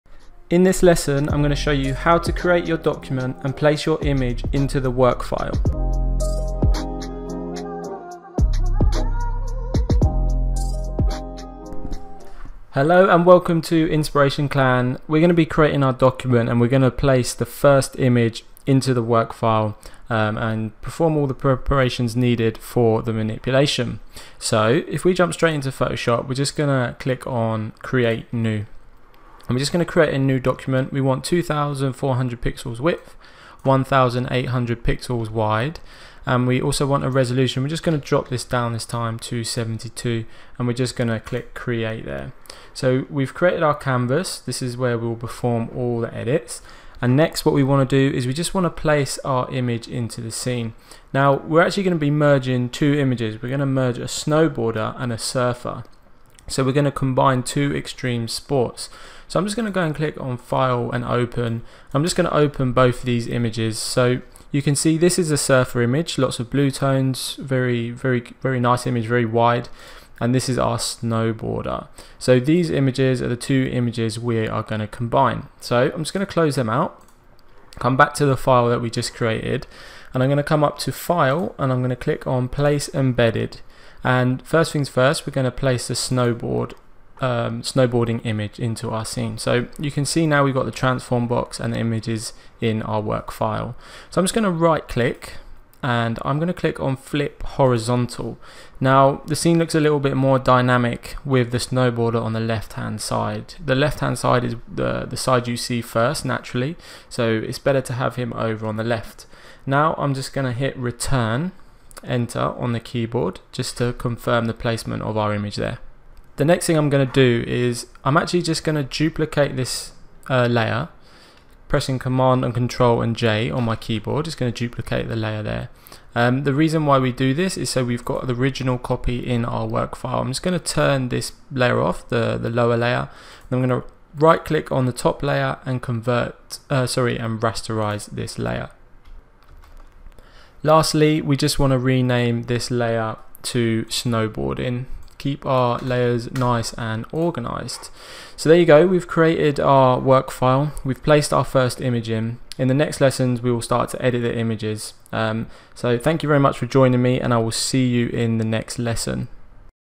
Part 1 - Create a Snowboard vs Surfer Photo Manipulation in Photoshop
In this lesson we begin the creation of the snowboard vs surfer photo manipulation in Adobe Photoshop